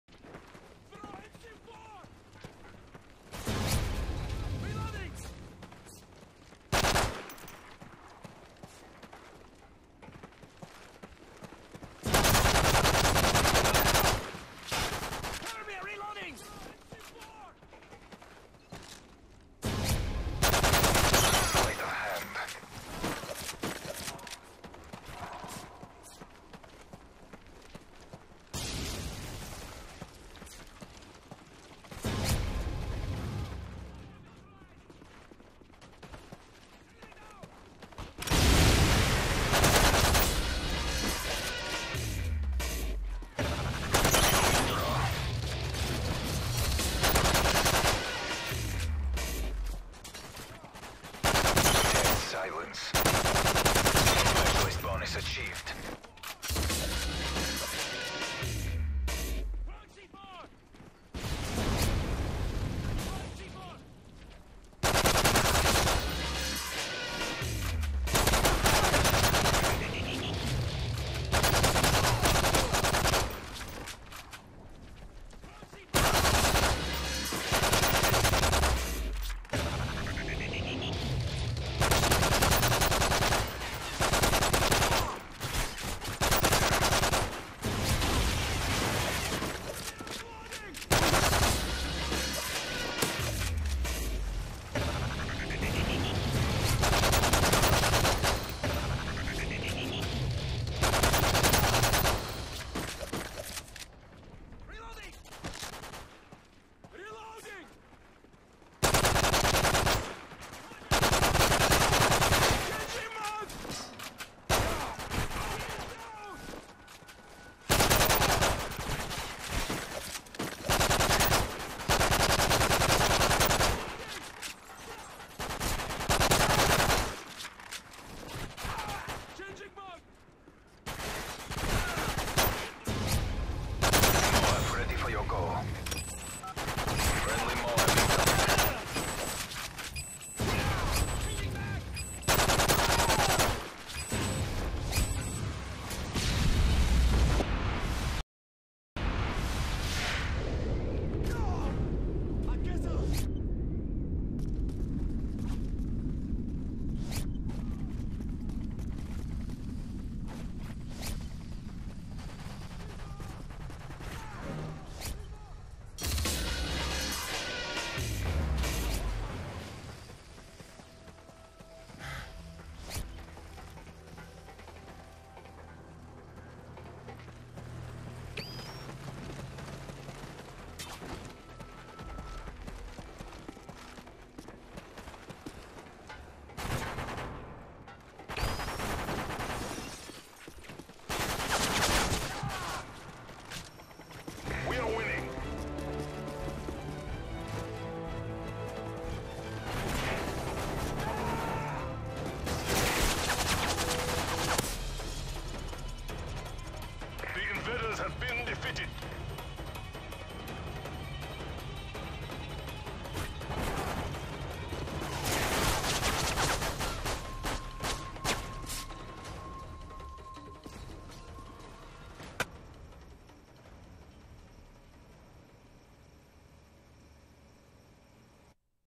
MOAB on Infected + game ending killcam (40-2)
My first MOAB so I was kind of excited. Sure it's not me running around Dome quickscoping and getting a MOAB in 30 seconds or some shit but I was happy.

I don't know what happened in the editing but before the bomb goes off it somehow got the begining of the video for about a second. Who knows.